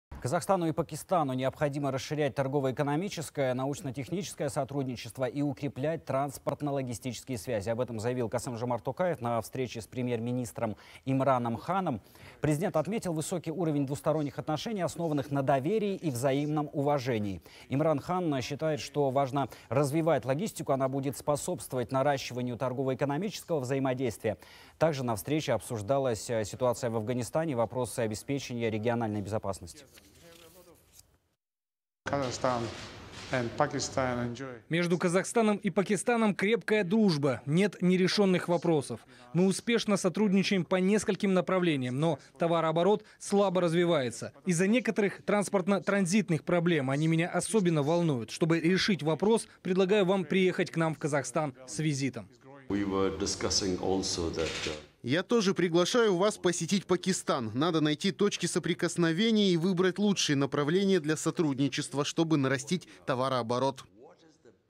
Двусторонние отношения Казахстана и Пакистана обсудили главы государств
Казахстану и Пакистану необходимо расширять торгово-экономическое, научно-техническое и культурное сотрудничество и укреплять транспортно-логистические связи.

Об этом заявил Касым-Жомарт Токаев на встрече с премьер-министром Имраном Ханом. Президент отметил высокий уровень двусторонних отношений, основанных на доверии и взаимном уважении. Имран Хан считает, что важно развивать логистику, она будет способствовать наращиванию торгово-экономического взаимодействия. Также представители сторон обменялись мнениями по ситуации в Афганистане и обсудили вопросы обеспечения региональной безопасности.